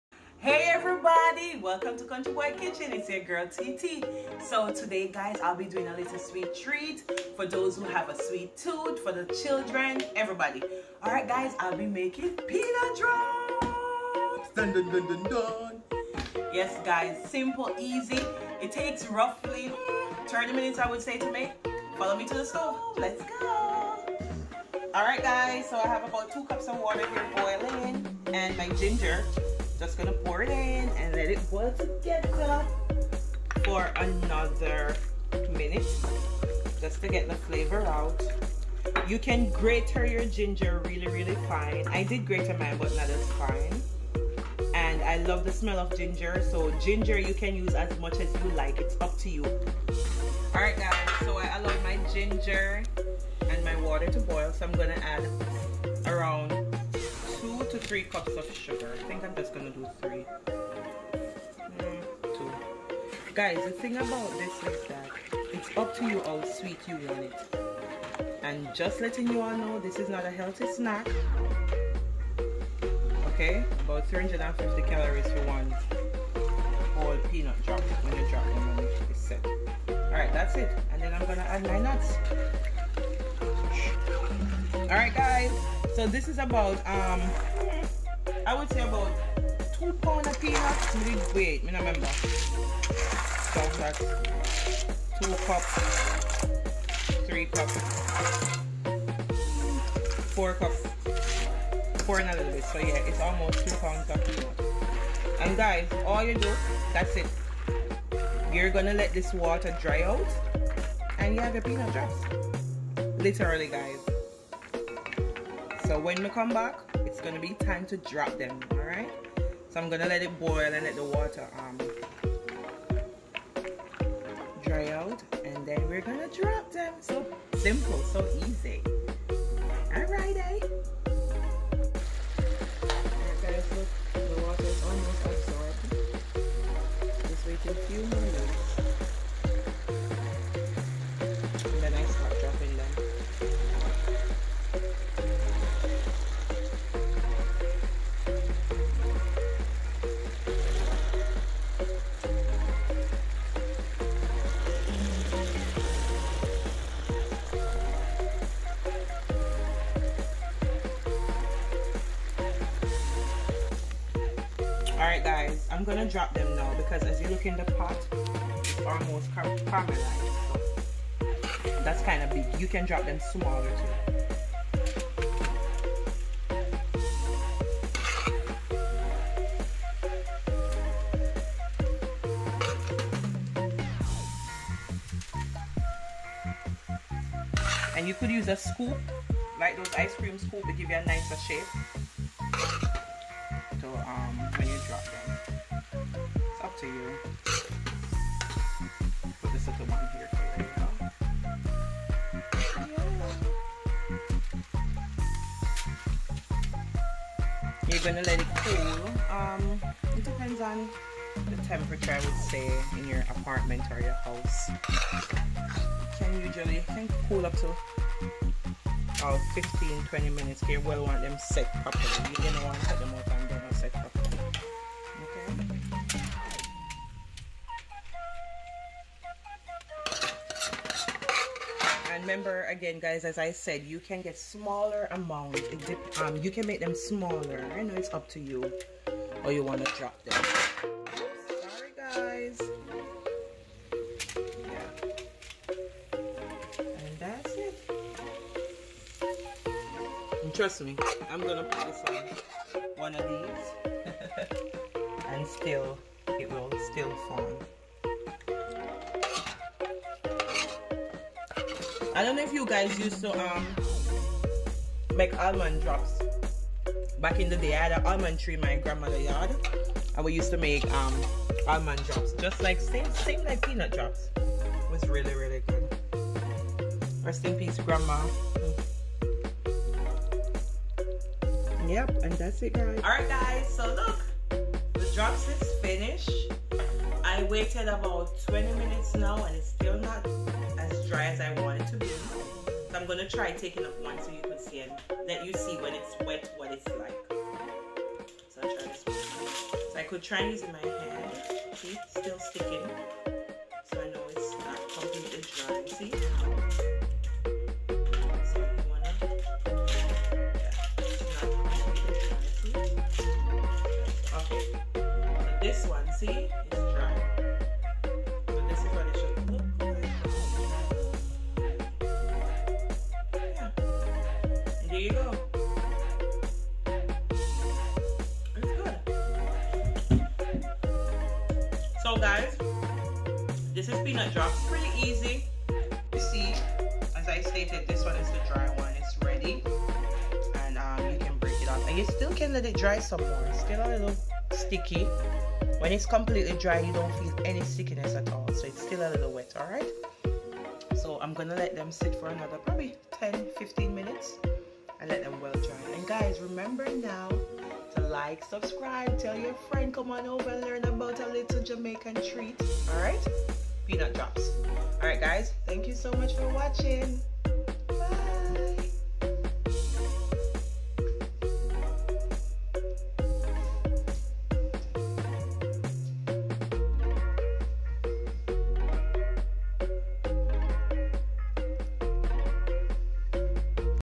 Jamaican Peanut drops
making something  sweet  for the kids  blessed
part of our culture